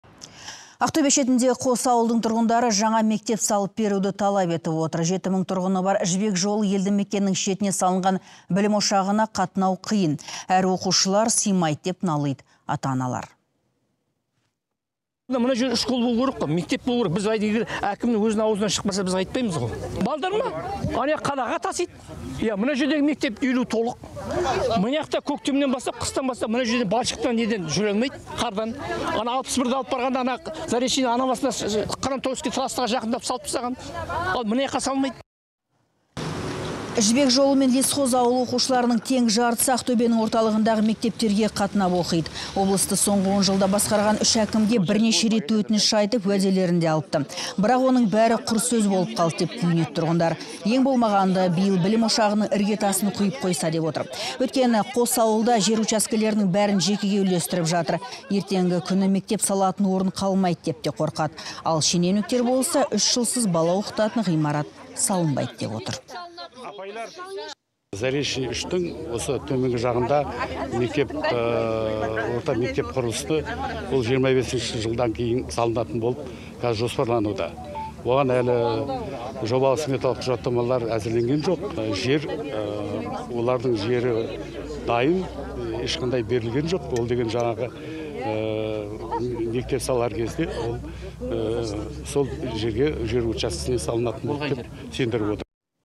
Жібек жолы мен Лесхоз тұрғындары мектеп салып беруді талап етеді – Ақтөбе
Ақтөбе шетіндегі қос ауылдың тұрғындары жаңа мектеп салып беруді талап етіп отыр. Жеті мың тұрғыны бар Жібек жолы елдімекеннің шетіне салынған білім ошағына қатынау қиын әрі оқушылар сыймайды деп налиды ата-аналар.